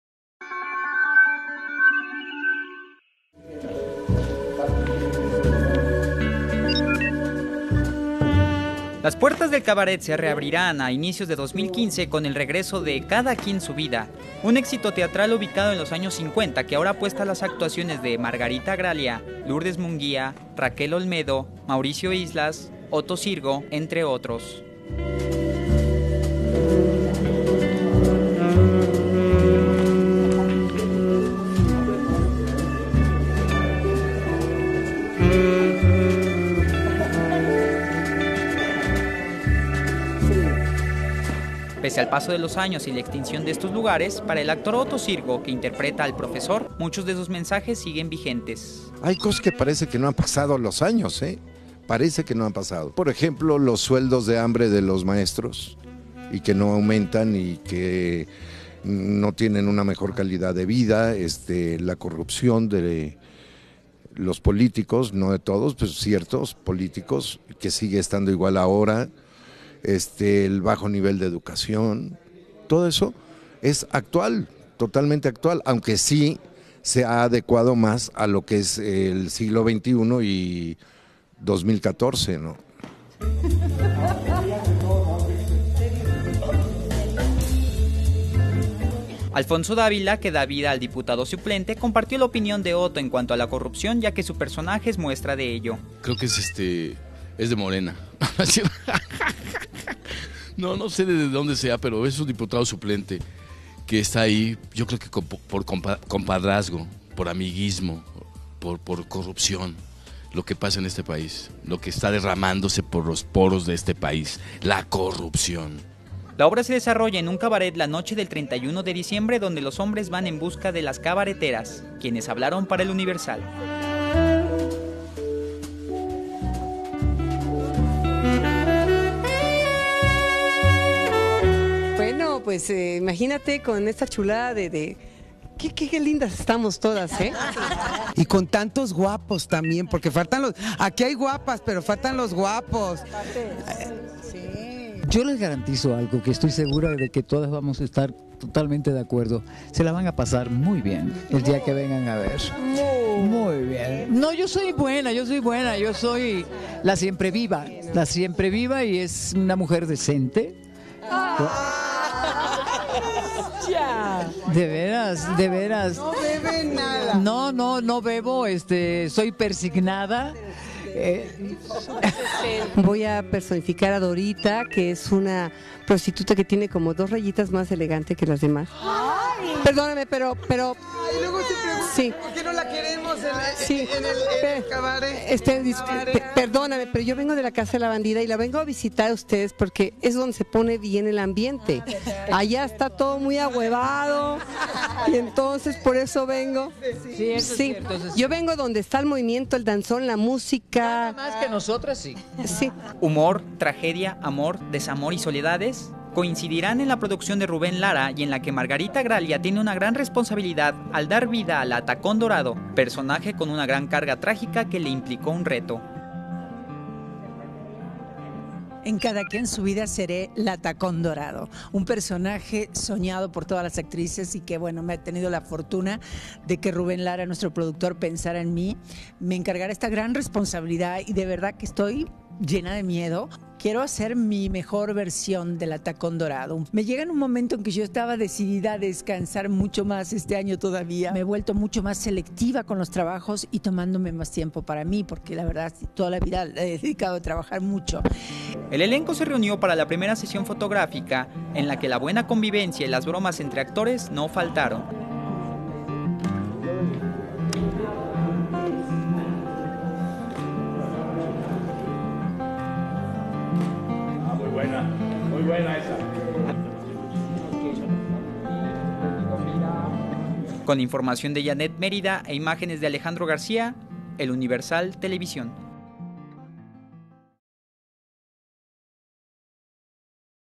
"Cada quien su vida" regresa a la puesta en escena
Margarita Gralia estelarizará la obra con el personaje de "La Tacón Dorado"